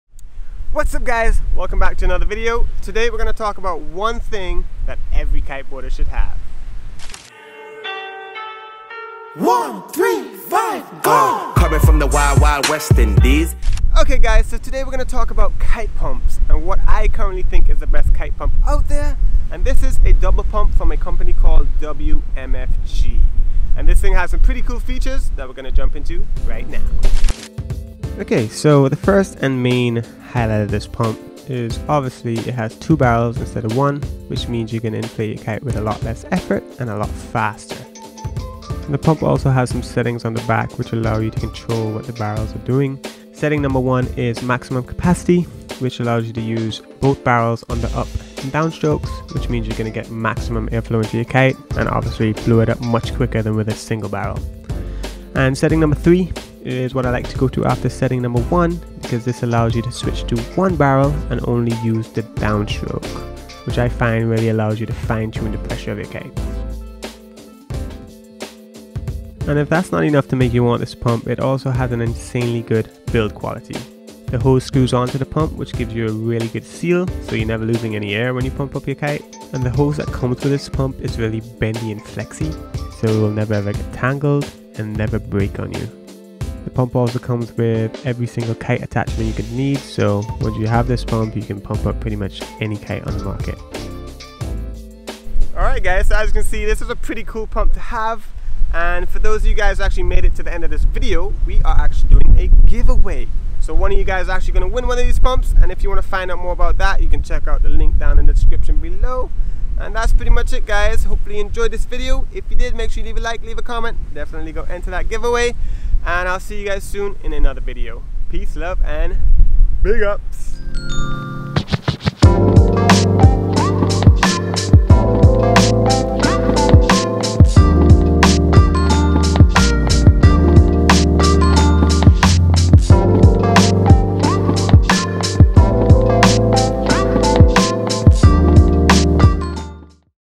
One Thing Every Kiteboarder Should Have! *Giveaway*
Need a new kiteboarding pump? This is the one you should get! This is hands down the best kite pump money can buy.

More Fun On INSTAGRAM!

CAMERA GEAR I Use To Make YOUTUBE VIDEOS -

Dakine (Travel Bags & Kite Harnesses)